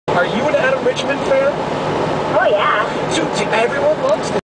Jesse, Golden Retriever and his flappy flews
Damn. those are some flappy flews!
Our dog Jesse loves to stick his head out the window of a moving vehicle. Good for a laugh when you look in your side view mirror.